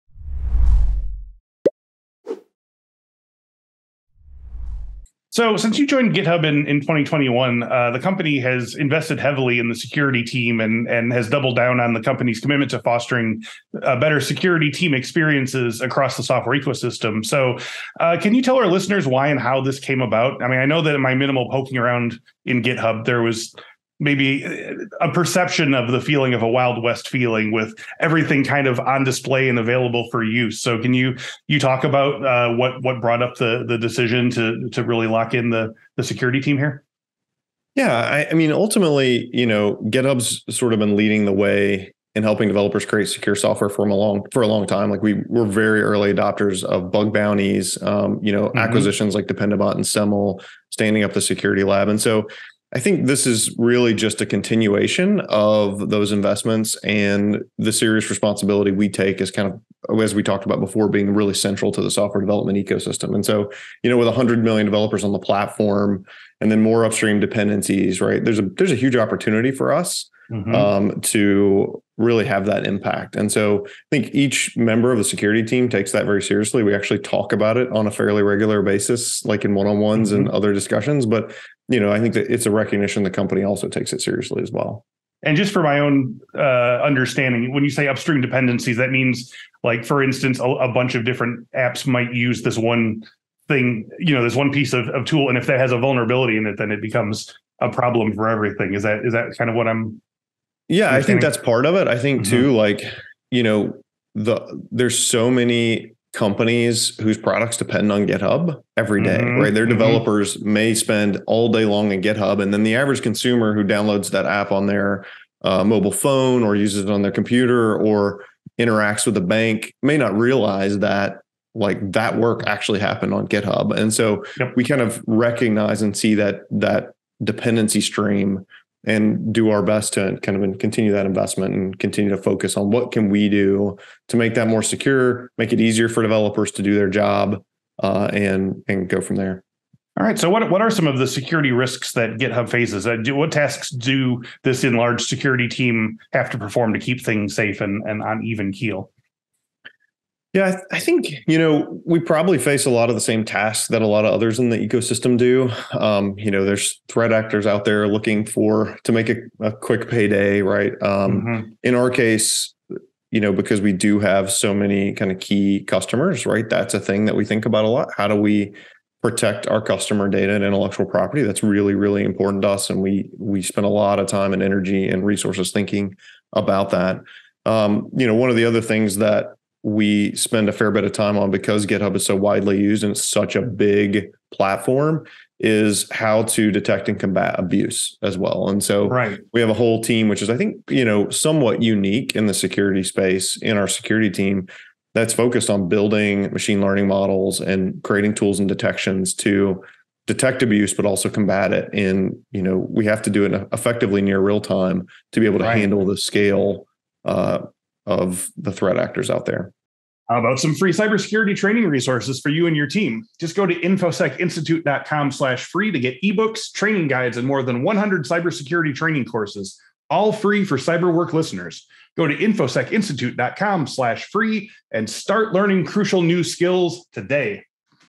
Meet GitHub’s security team | Cyber Work Podcast
Jacob DePriest, the VP and deputy chief security Officer at GitHub, talks about joining the company in 2021 and what its security team is like, as well as GitHub's security issues, how they are similar to other developer platforms and how they are different.